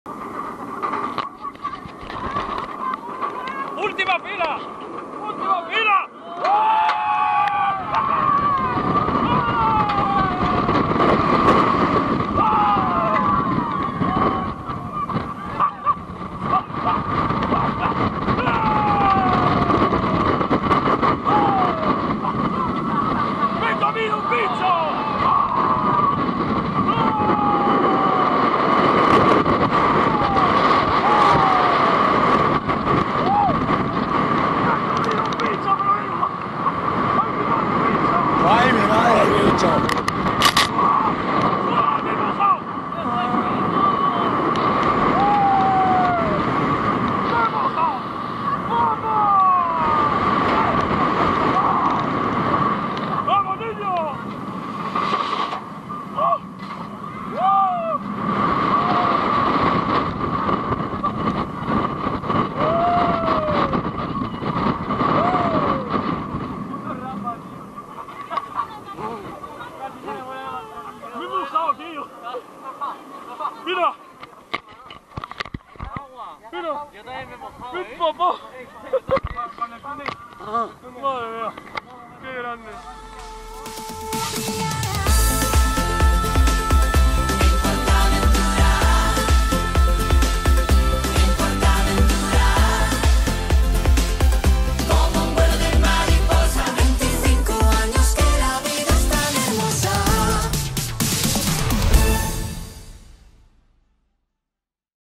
ME COMO un BICHO en SHAMBHALA!! | Trailer Vlog PortAventura 25+1 Aniversario
Hola a todos!! Para ir abriendo boca, y nunca mejor dicho, os traigo un onride de Shambhala en PortAventura dónde sufro un percance con un bicho... y además el tráiler del super vlog del viaje veraniego al resort.

Muy pronto no os perdáis estos vídeos y otra serie de acontecimientos que están por venir...